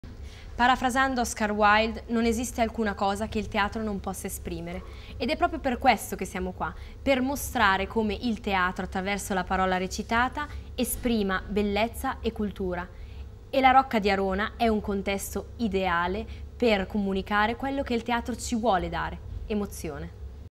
La Rocca di Arona luogo di teatro
Valentina Modini, una delle giovani volontarie del FESTIVAL DELLE DUE ROCCHE 2012, presenta la Rocca di Arona attraverso le parole di Oscar Wilde